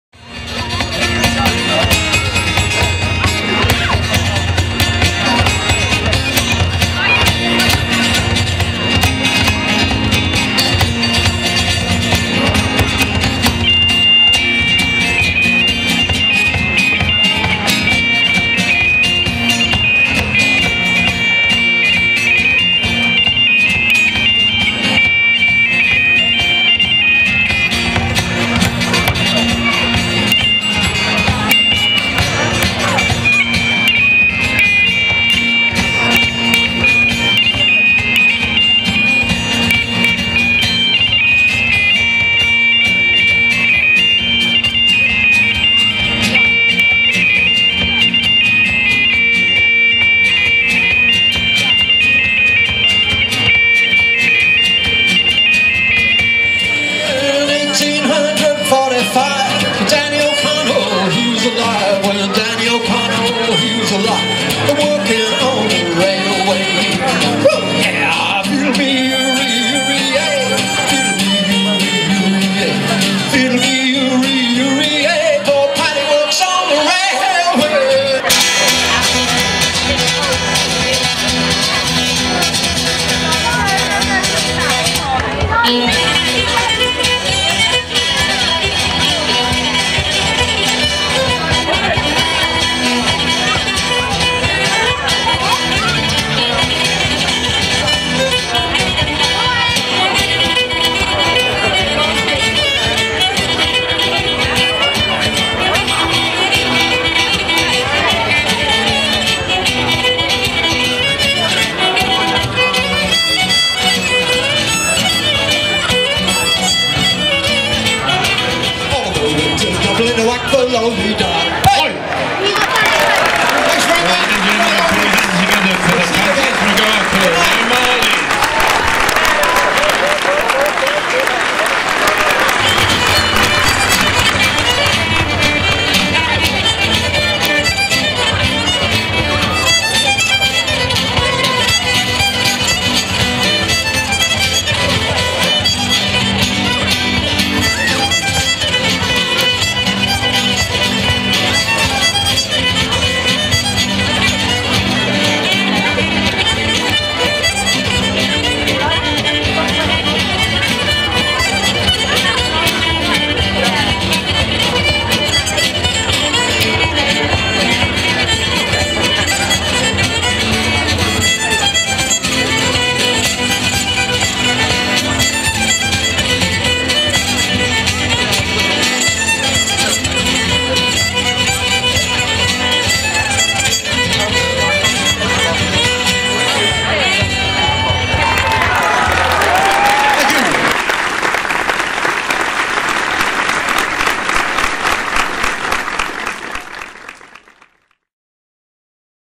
The O Marleys at Tiverton Balloon Festival 2012
Fantastic Exeter based Irish Band, The O Marleys, recorded live at Tiverton Balloon Festival 2012, They are based in the South West of the UK and feature John Fitzsimons, vocals guitar, Mac Moulton, Fiddle, pipes vocals, and James King, Bodhran and vocals.